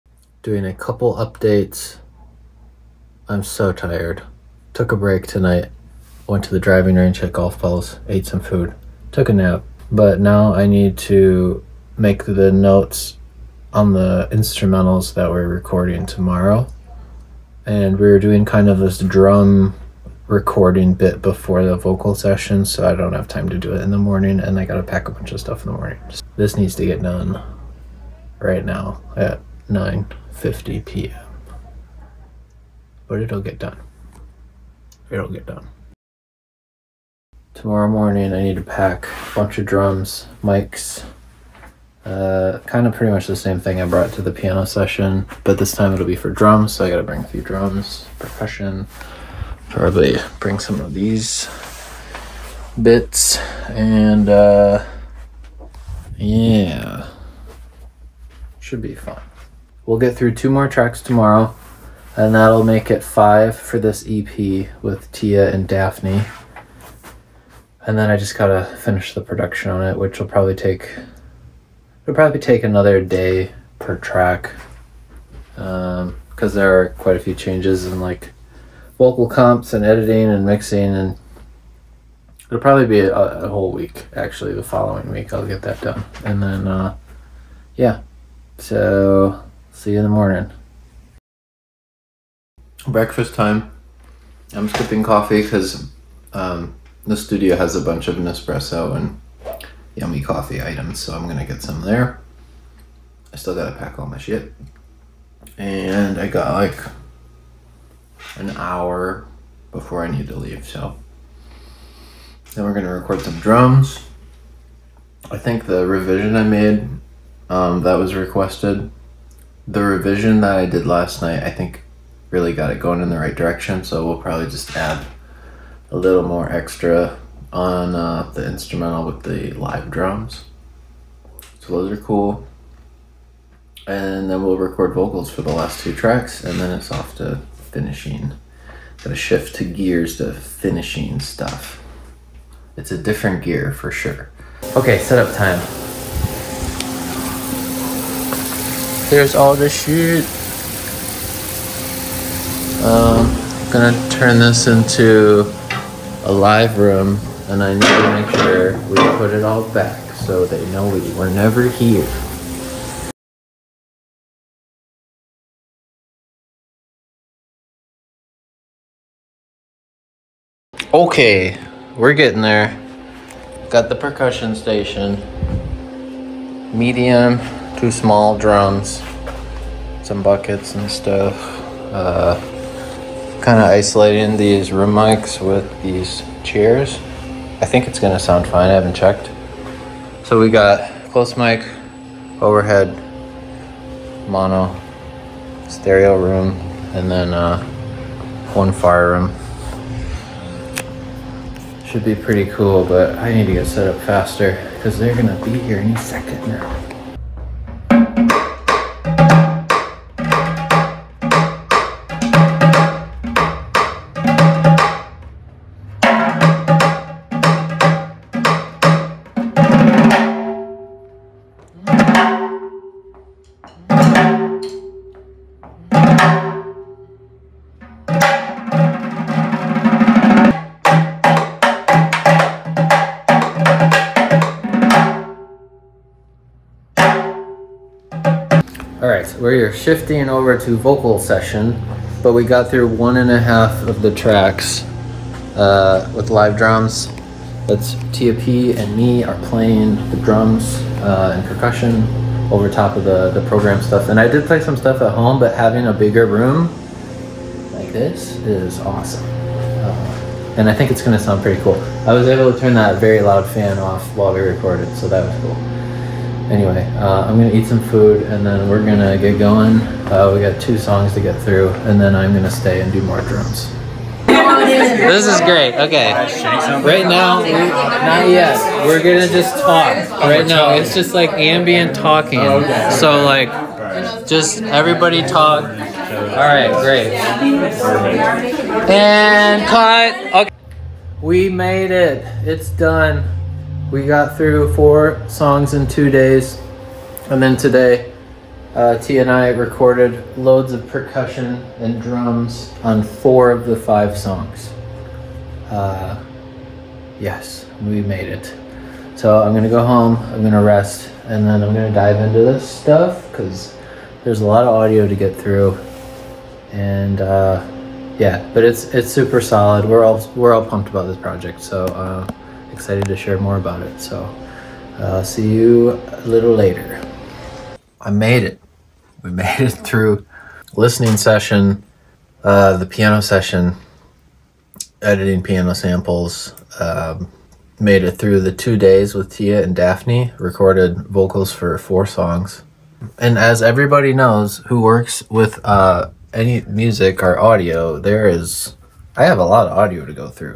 Composing and Producing Music in LA // Vlog 4
There are some projects I want to document here on my YouTube channel. Hope you enjoy this open and unscripted look into composing and producing music in Los Angeles. - Kevin

0:00 Re-arrangements before tomorrow's session
0:36 Prep for drum/perc recording
1:31 Skip coffee because studio has better coffee
2:24 Drum session setup
3:27 Tia P & Kevin DePree on drums!
3:58 Switching to vocal session
4:34 Position Music office helps record ambience
4:51 That's a wrap